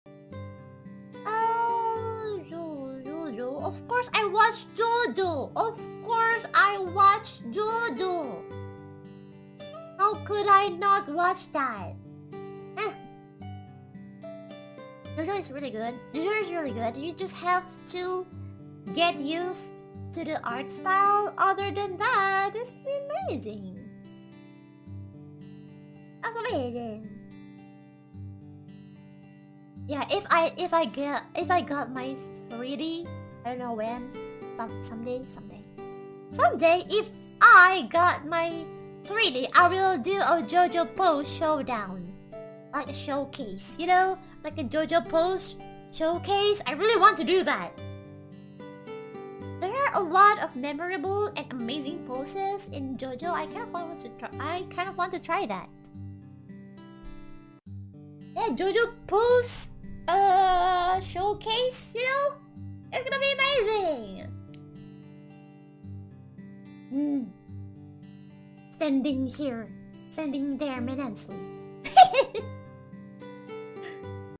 Iofi is a woman of culture
I'm looking forward to this. Yagoo give them 3D plz... they deserve more love

But it's true what she says, I wasn't sure of watching JoJos at first... but it is an amazing series indeed.

In this channel I'll upload spanish subbed clips of Vtubers so that everyone can enjoy them.